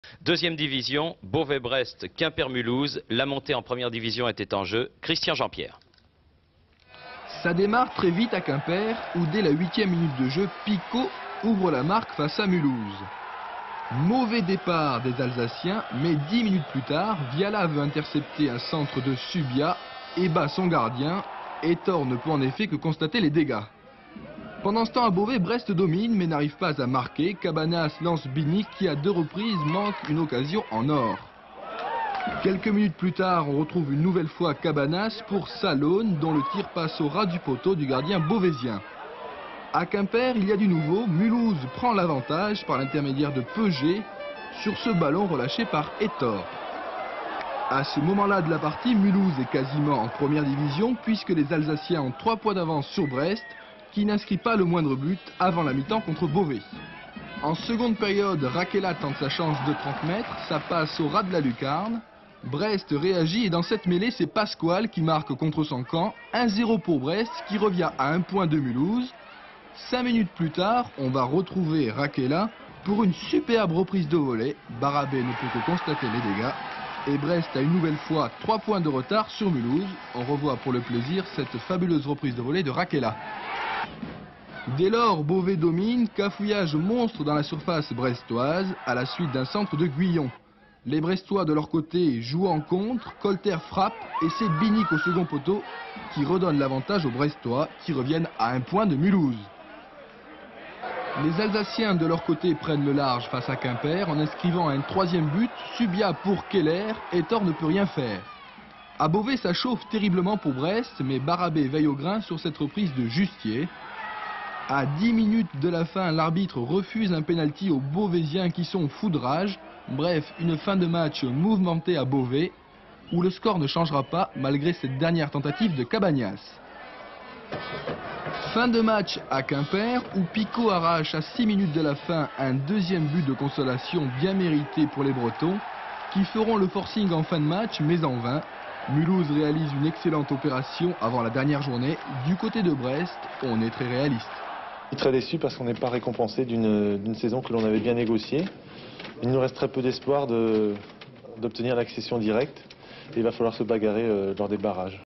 Quimper-Mulhouse et Beauvais-Brest 1988/89
Saison 1988/89 (D2)  - 33e journée - 9622 spectateurs - Arbitre : Veniel

QUIMPER 2 MULHOUSE 3
Picot (8e, 84e) / Viala (18e csc), Peuget (33e), Keller (73e)

Saison 1988/89 (D2)  - 33e journée - 2153 spectateurs - Arbitre : Lavis

BEAUVAIS 1 BREST 2
Rakela (59e) / Pascual (51e csc), Binic (70e)